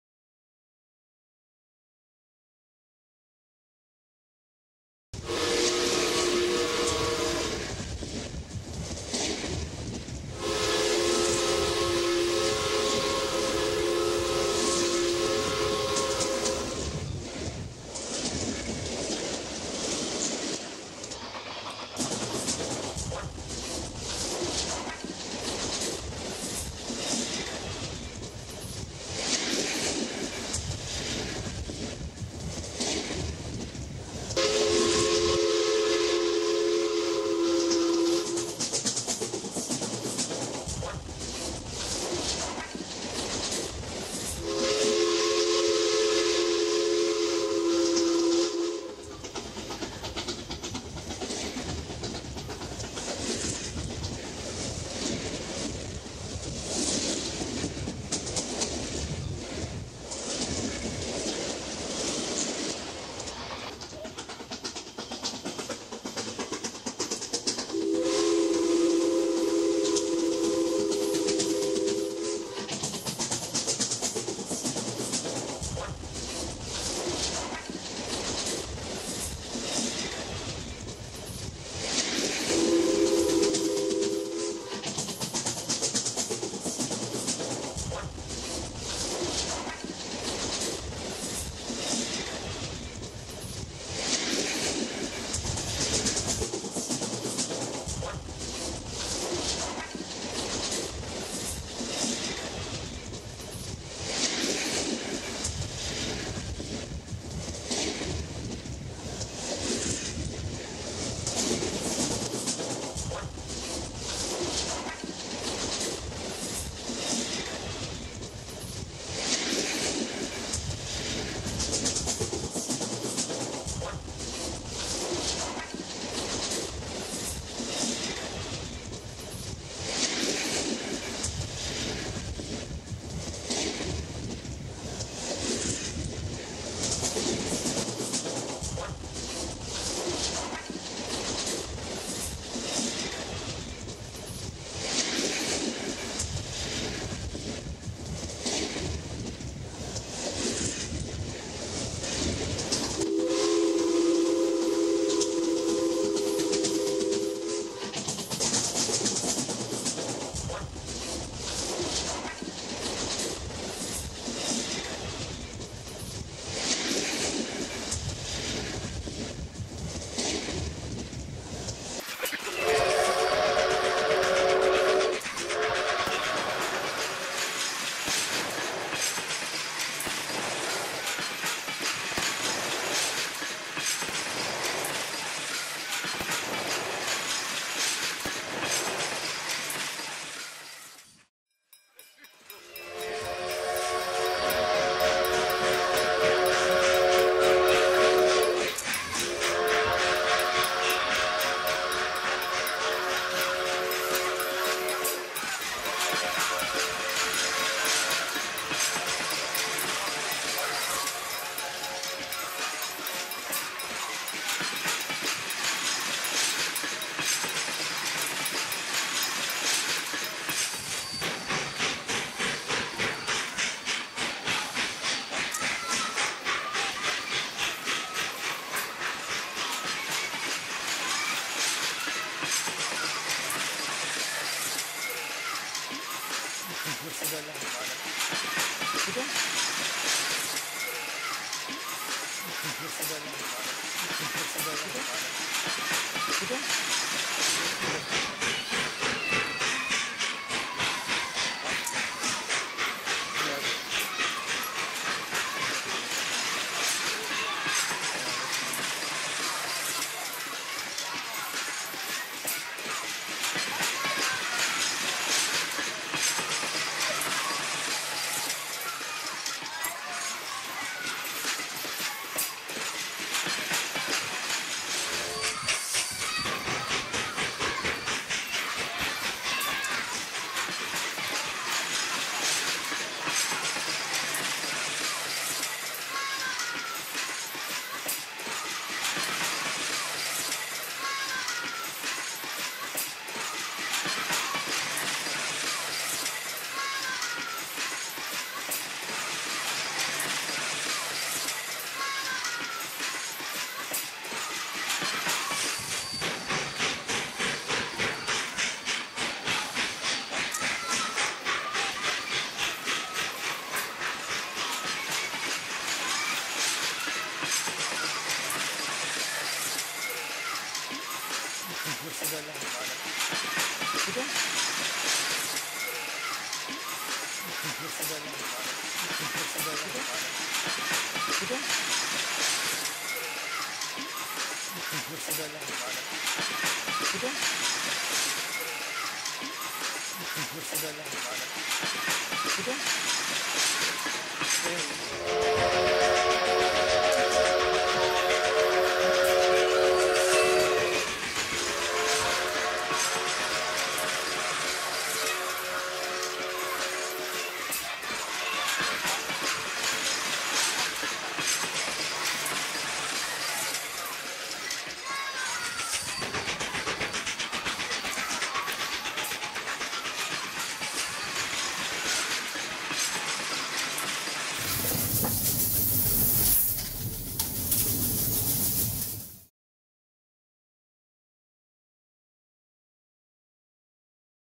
OUT IN THE WILD WEST G SCALE RAILROAD
OUT IN THE WILD WEST WITH ARCKY AND THE LOST PREVENTION TEAM ROUNDING UP ALL OF THE DONATIONS.

ITEMS USED IN THE VIDEOS.

LARGE SIZE STARWAR FIGURE AND LARGE SCALE TOY SOLDIERS
OBTAINED FROM ARC THRIFT STORES IN 2019

NEWRAY G SCALE EUROPEAN TRAIN SET ORDERED FROM OVERSEASE.

Added additional cars came with another G scale train set that was recently purchased from Home depot and ebay. 2020

Filmed at
A.S. BLIND MADE PRODUCTION MULTI MEDIA ART STUDIO.